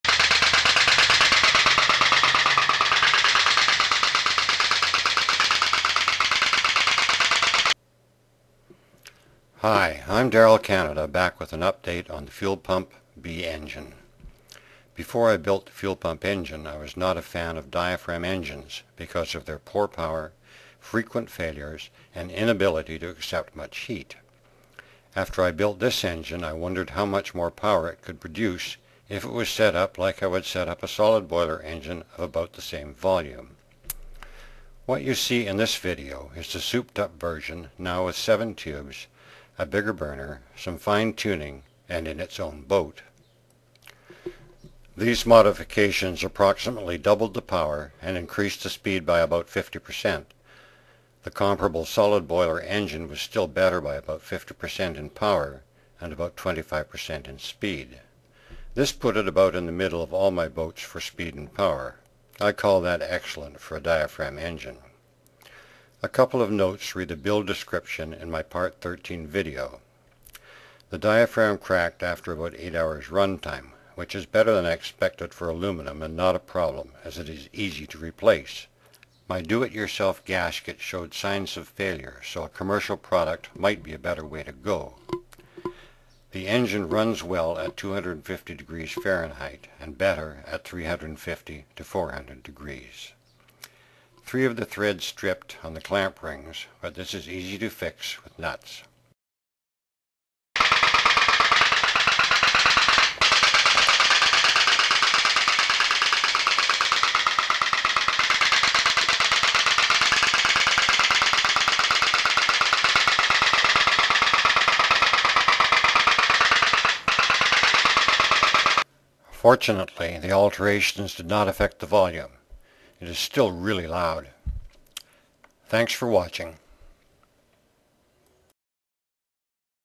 Pop Pop Boat Loud Jack Black-YT.mpg
Modifying a putt putt engine for more speed and power.